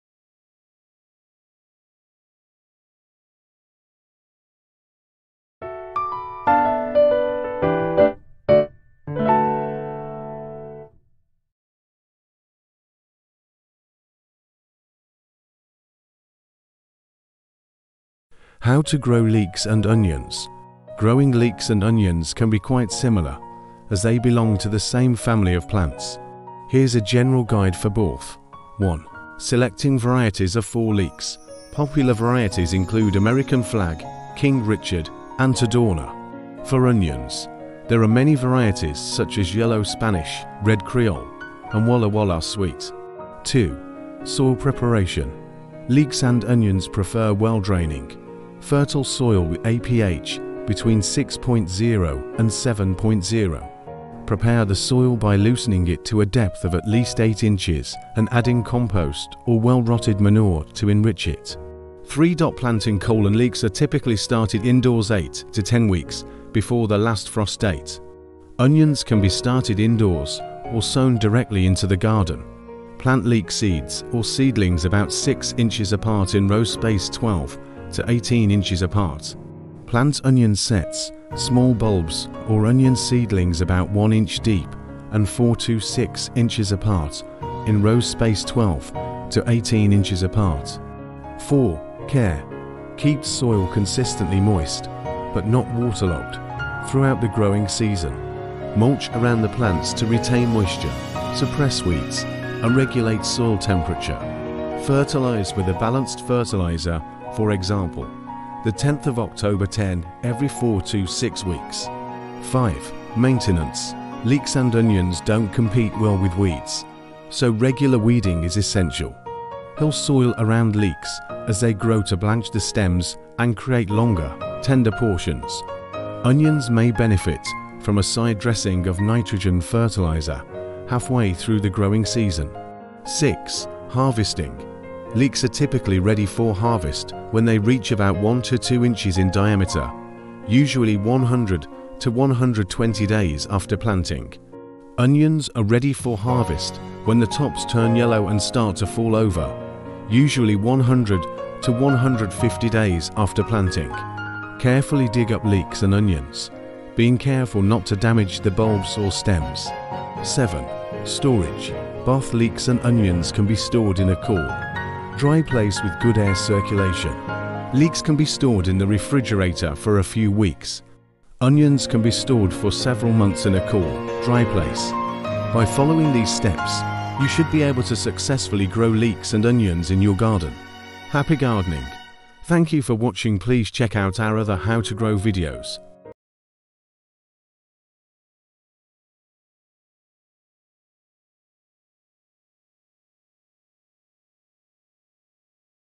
How And Where To Grow Onions And Leeks
Growing leeks and onions can be quite similar, as they belong to the same family of plants. Here's a general guide for both:

**1. Selecting Varieties:**
   - For leeks, popular varieties include American Flag, King Richard, and Tadorna.
   - For onions, there are many varieties such as Yellow Spanish, Red Creole, and Walla Walla Sweet.

**2. Soil Preparation:**
   - Leeks and onions prefer well-draining, fertile soil with a pH between 6.0 and 7.0.
   - Prepare the soil by loosening it to a depth of at least 8 inches and adding compost or well-rotted manure to enrich it.

**3. Planting:**
   - Leeks are typically started indoors 8-10 weeks before the last frost date. Onions can be started indoors or sown directly into the garden.
   - Plant leek seeds or seedlings about 6 inches apart in rows spaced 12-18 inches apart.
   - Plant onion sets (small bulbs) or onion seedlings about 1 inch deep and 4-6 inches apart in rows spaced 12-18 inches apart.

**4. Care:**
   - Keep the soil consistently moist, but not waterlogged, throughout the growing season.
   - Mulch around the plants to retain moisture, suppress weeds, and regulate soil temperature.
   - Fertilize with a balanced fertilizer (e.g., 10-10-10) every 4-6 weeks.

**5. Maintenance:**
   - Leeks and onions don't compete well with weeds, so regular weeding is essential.
   - Hill soil around leeks as they grow to blanch the stems and create longer, tender portions.
   - Onions may benefit from a side-dressing of nitrogen fertilizer halfway through the growing season.

**6. Harvesting:**
   - Leeks are typically ready for harvest when they reach about 1-2 inches in diameter, usually 100-120 days after planting.
   - Onions are ready for harvest when the tops turn yellow and start to fall over, usually 100-150 days after planting.
   - Carefully dig up leeks and onions, being careful not to damage the bulbs or stems.

**7. Storage:**
   - Both leeks and onions can be stored in a cool, dry place with good air circulation.
   - Leeks can be stored in the refrigerator for a few weeks.
   - Onions can be stored for several months in a cool, dry place.

By following these steps, you should be able to successfully grow leeks and onions in your garden. Happy gardening!